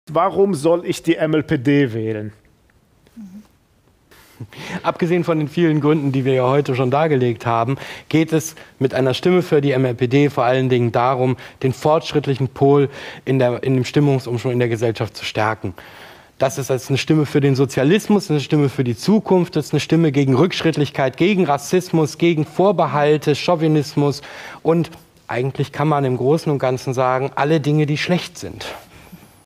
Fritz Ullmann: Warum MLPD wählen
5 Gründe, die MLPD zu wählen!
1. Kein Kanzlerkandidat ist wählbar - wähle Marxisten-Leninisten, Kapitalismuskritik im Original! Keine Stimme der Rechtsentwicklung und dem Krisenchaos.

2. Diesmal keine halben Sachen! Das große Übel des Kapitalismus bekämpft man nicht mit kleinen Übeln des Opportunismus. Auch rot-rot-grün wird den Kapitalismus nicht verbessern können.

3. Wähle die einzige Arbeiterpartei, mit der Werkzeugmechanikerin Gabi Fechtner an der Spitze! Für die 30-Stunden-Woche bei vollem Lohnausgleich, Weg mit Hartz IV, für ein Rentenniveau von 70% des bisherigen Netto-Verdienstes.

4. Wähle die einzige Partei, deren Programm die globale Umweltkatastrophe verhindern kann!

5. CSU-Söder zum Trotz: Stärke jetzt erst recht mit deiner Stimme den Kampf um den echten Sozialismus! Gib auch am Wahlsonntag dem Antikommunismus keine Chance.